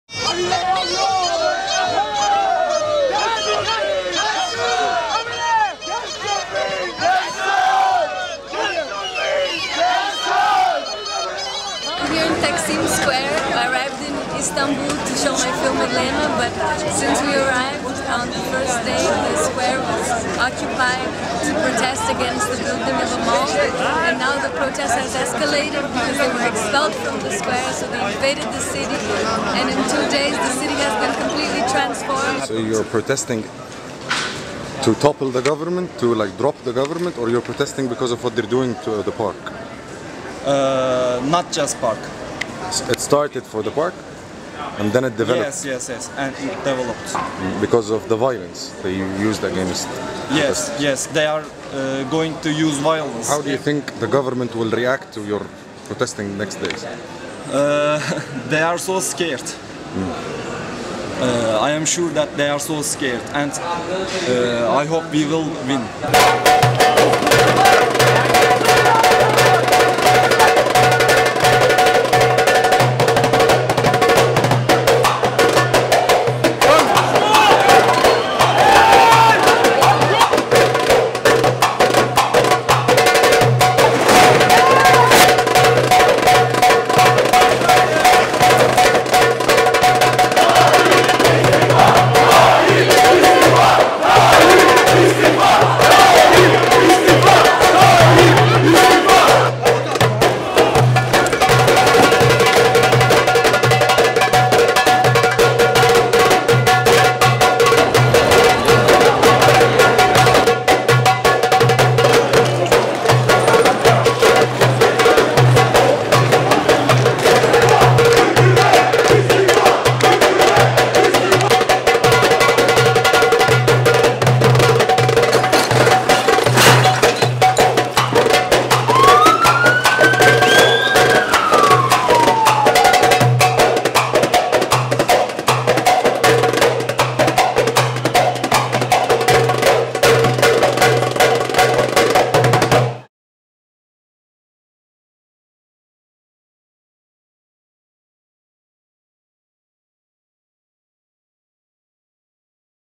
ELENA and the Protests in Istanbul: Beyond the Tree, the Forest
June 2, 2013  -  Coverage from Taksim Square - Busca Vida Films
Images - Petra Costa and Muhammed Hamdy
Editing - David Senigo